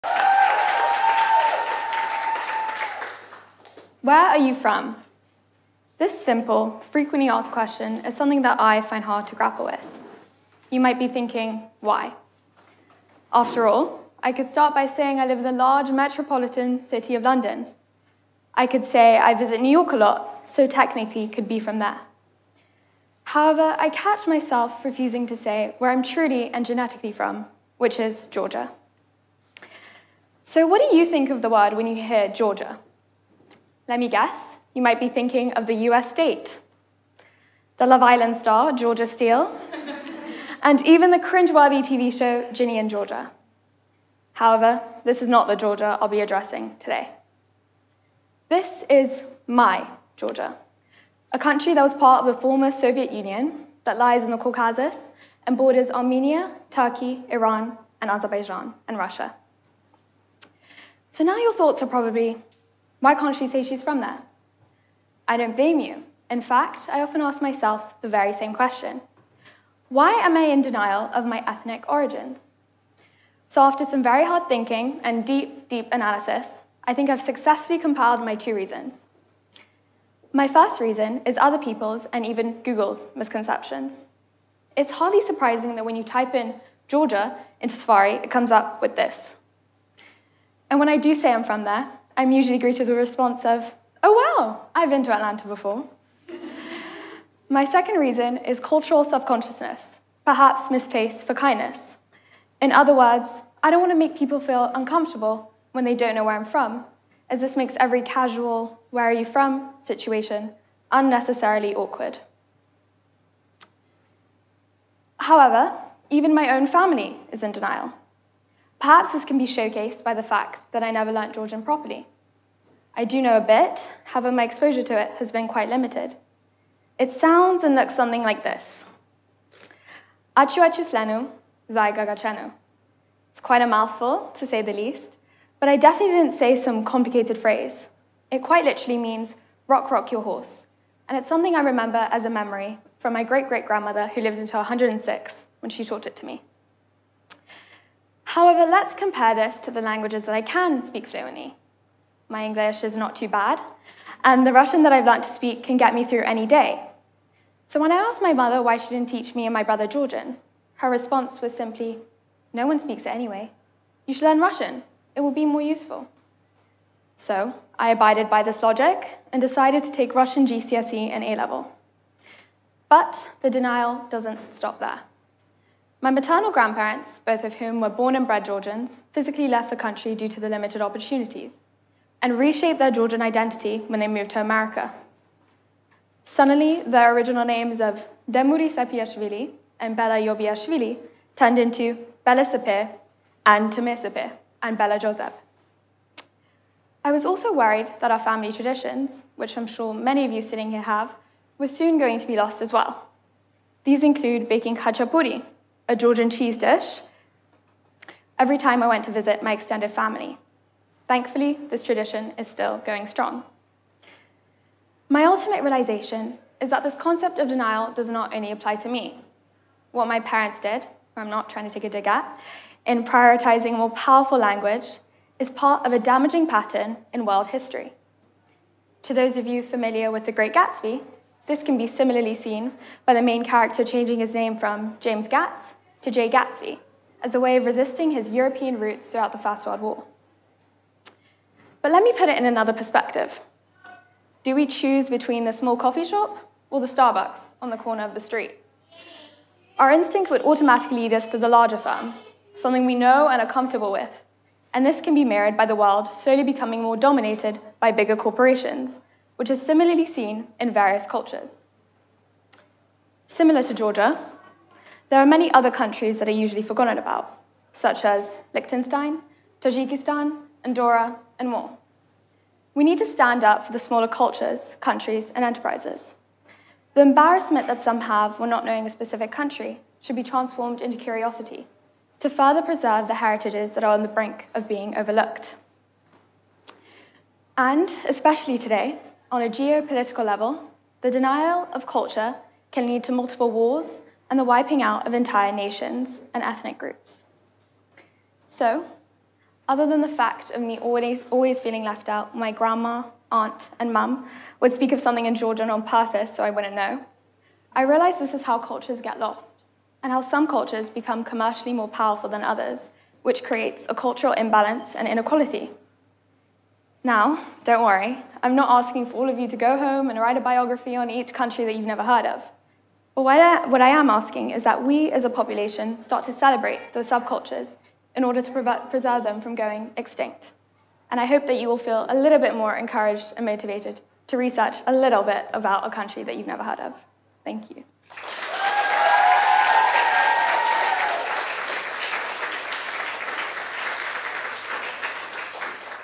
Nobody Knows Where I Am From | Ariana Barinstein | TEDxFrancisHollandSchoolSloaneSquare
Have you ever struggled with your cultural identity? Do you try and prioritise another culture over your own just because it is more known and widespread? This talk will explore how cultural inequality exists in everyone’s lives and how we can start to combat it to preserve the standing of small countries. Ariana is a student at Francis Holland School, Sloane Square, with a keen interest in studying International Business/Economics, along with Eastern European studies. This talk was given at a TEDx event using the TED conference format but independently organized by a local community.